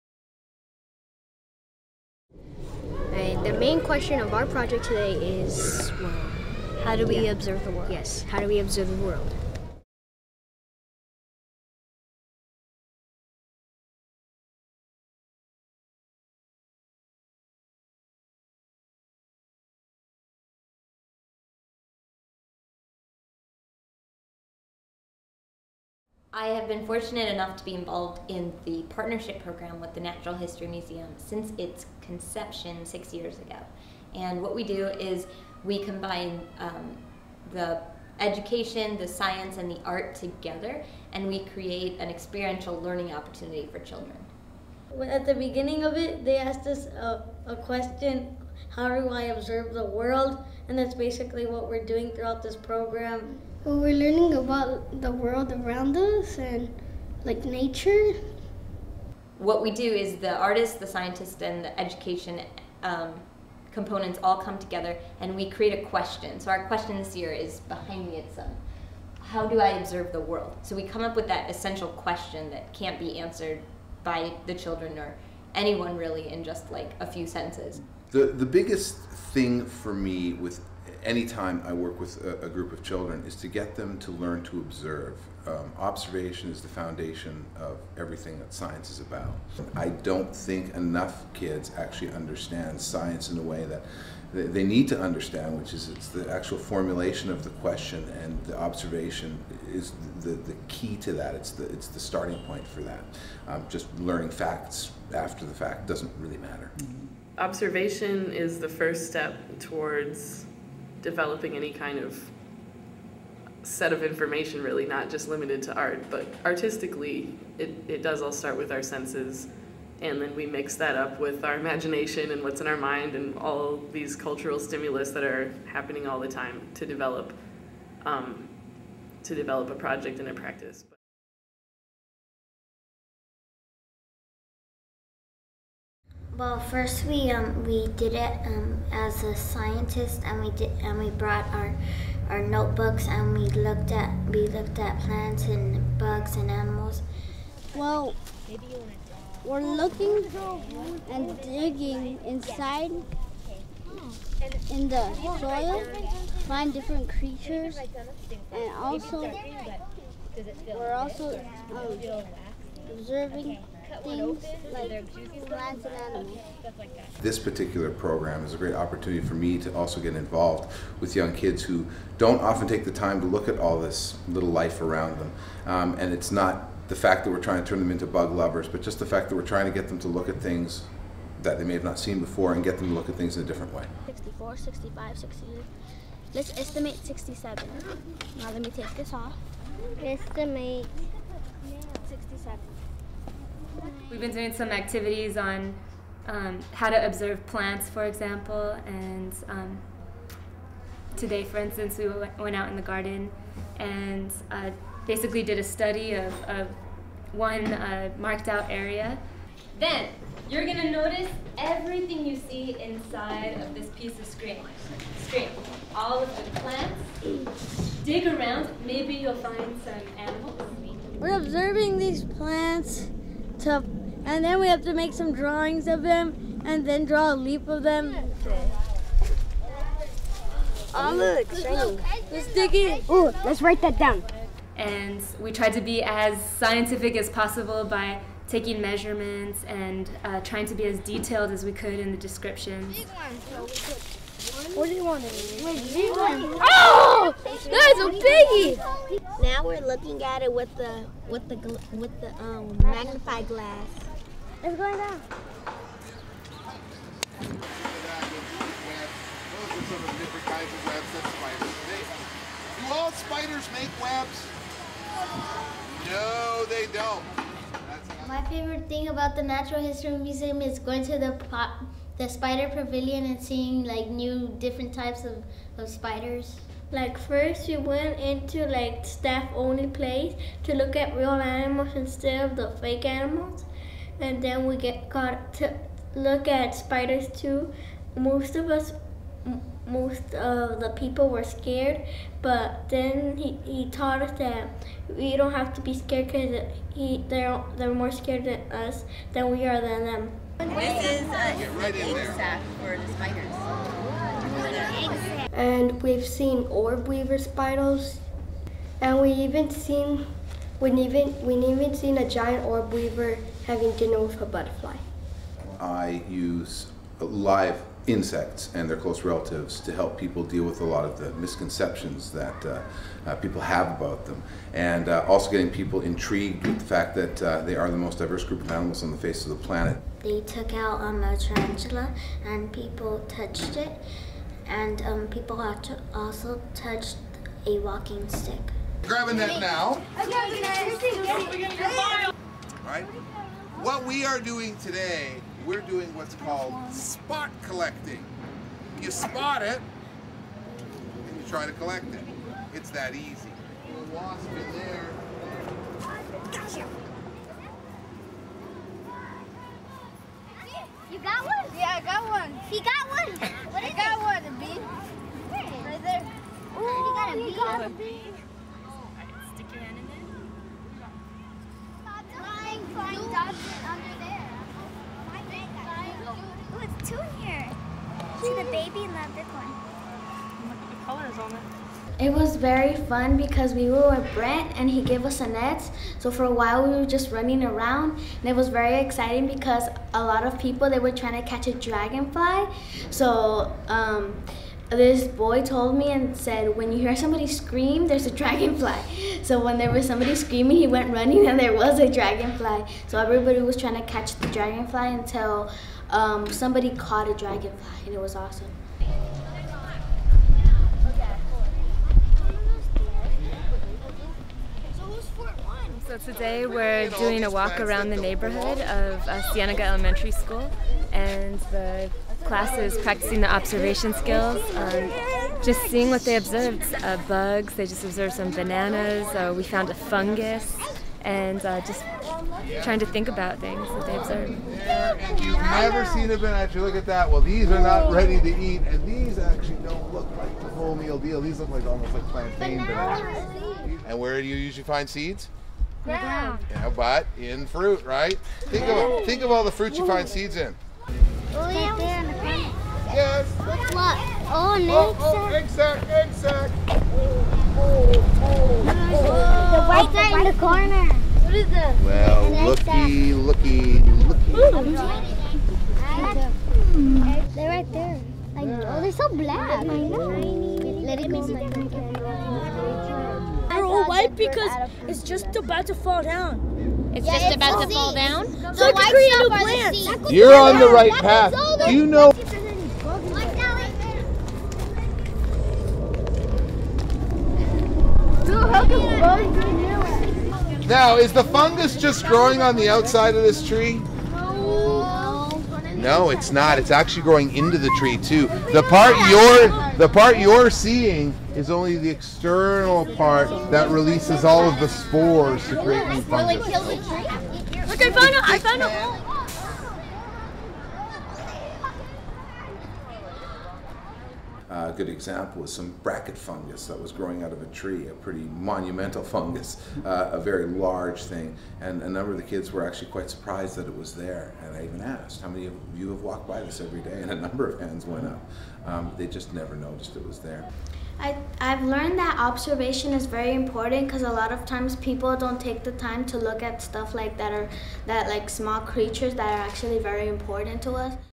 Inter/Act: How do I observe my world? (part 1)
In an eight-week, project-based-learning assignment, 4th and 5th graders at Cienega Elementary School learned to observe and question the natural world through the lens of both scientist and artist.  They wrote, sketched, and documented their observations, as they explored the grounds in and around their neighborhood.

Using photography as a medium, the students deepened their awareness of the natural world by studying with both Museum scientists and an artist/photographer.  The students participating in this after-school club produced a series of photographs, poetry and journal descriptions aimed at answering the essential question:  How do I observe my world?